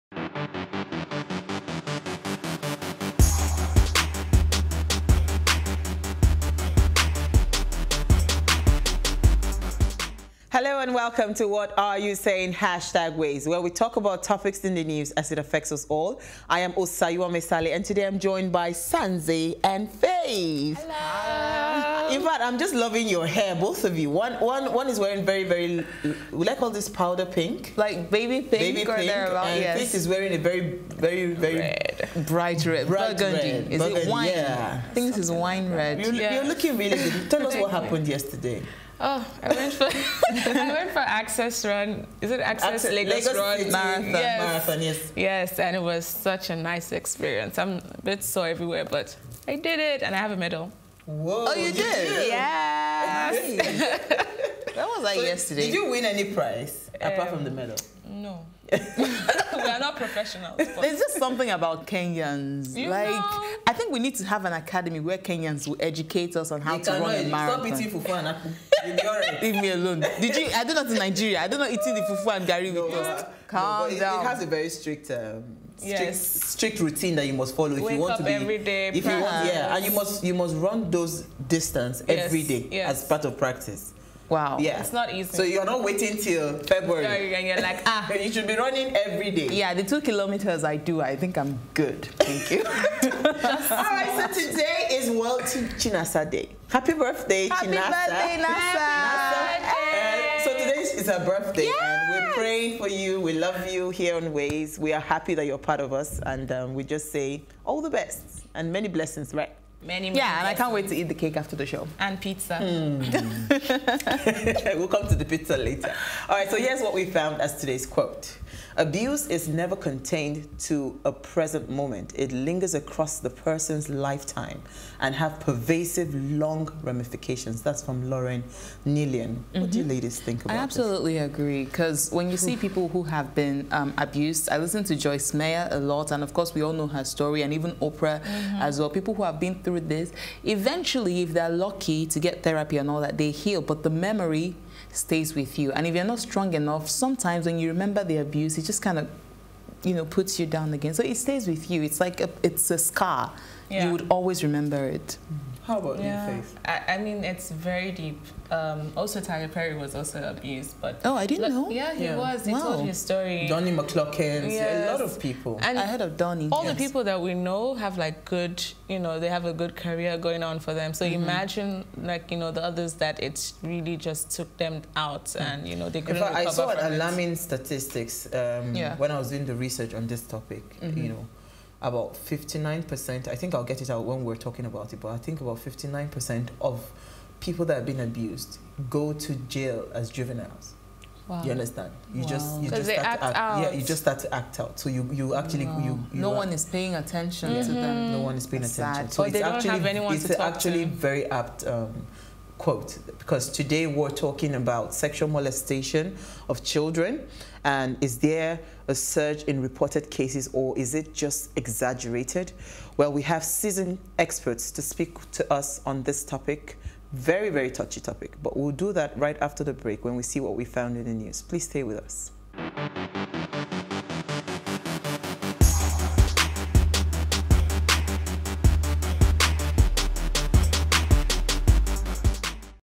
Abuse Comes With A Lifetime Of Long-term Consequences
"Abuse is never contained to a present moment, it lingers across a person's lifetime and has pervasive long-term ramifications" - Lorraine Nilon.